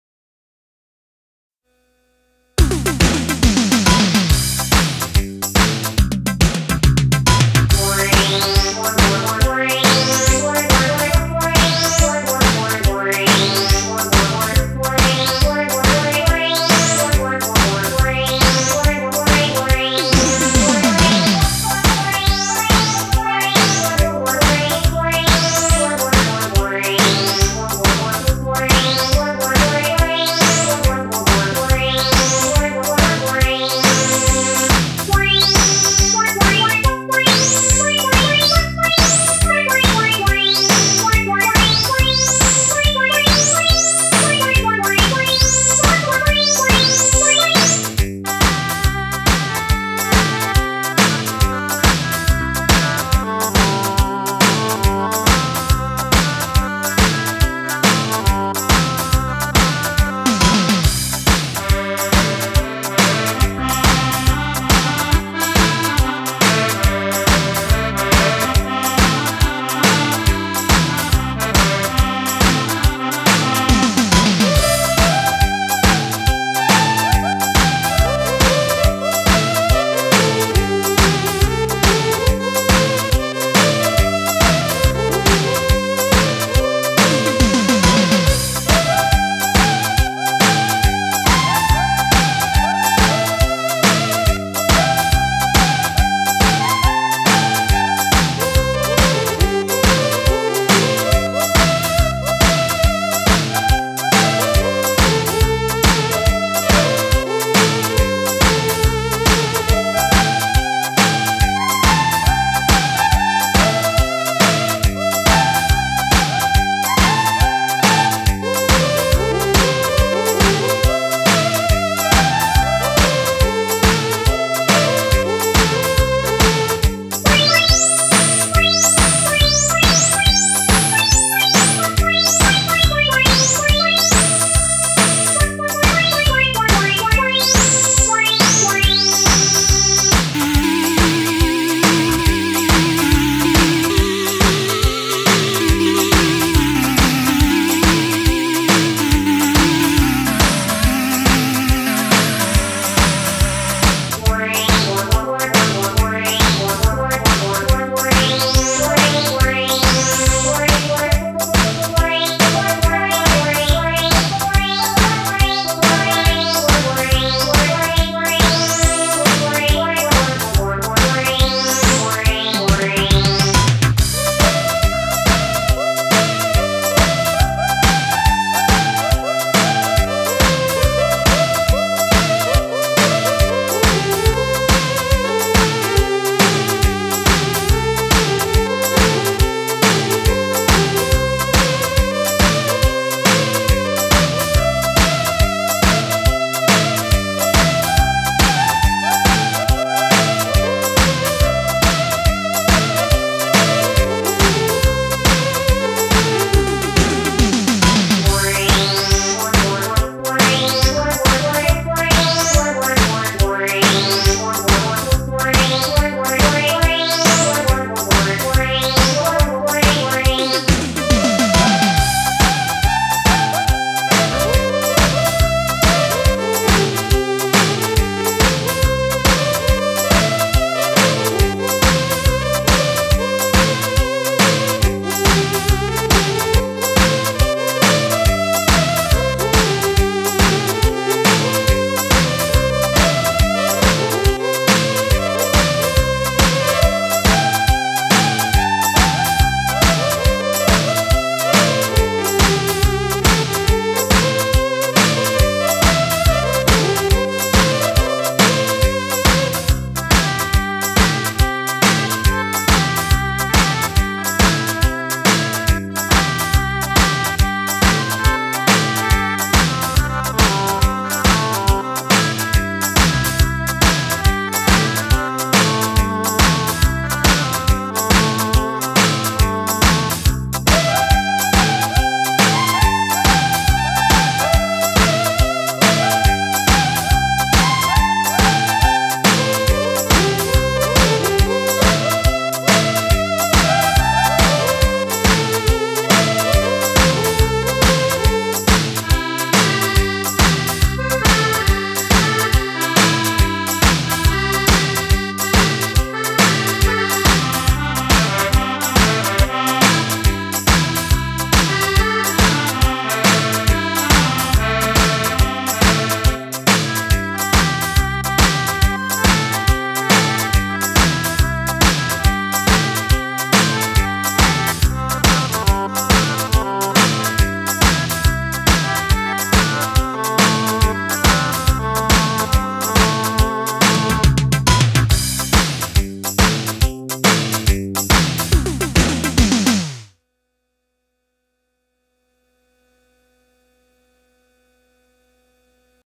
민요 매들리 / 샛강역에서 관악산까지. 신림선 개통
전자 오르간 연주 # 김. 철  웅